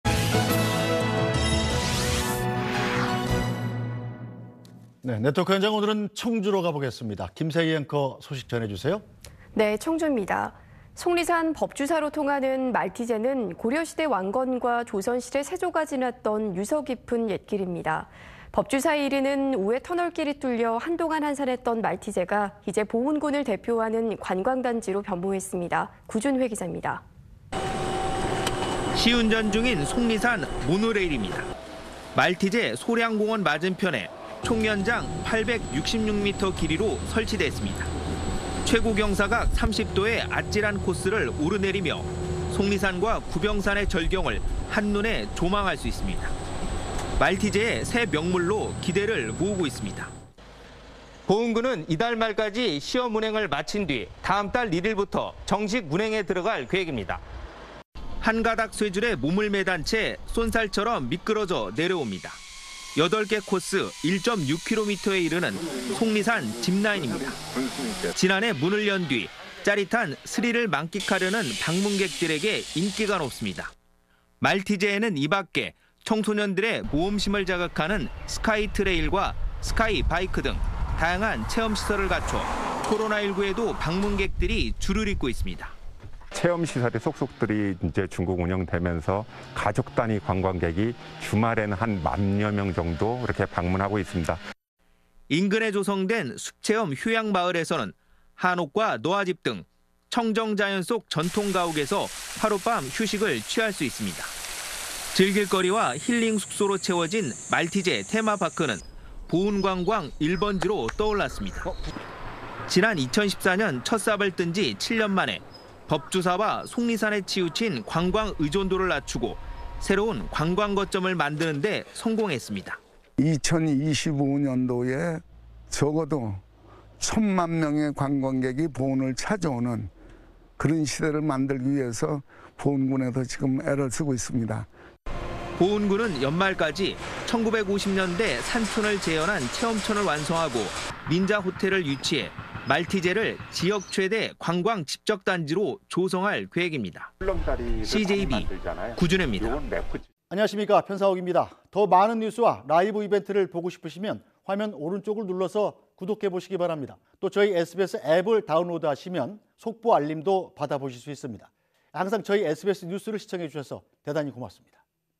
스릴도 힐링도 모두 만점!…속리산 말티재로 오세요 / SBS
속리산 법주사로 통하는 말티재는 고려 시대 왕건과 조선 시대 세조가 지났던 유서 깊은 옛길입니다. 법주사에 이르는 우회 터널길이 뚫려 한동안 한산했던 말티재가 이제 보은군을 대표하는 관광단지로 변모했습니다.
구준회 기자입니다.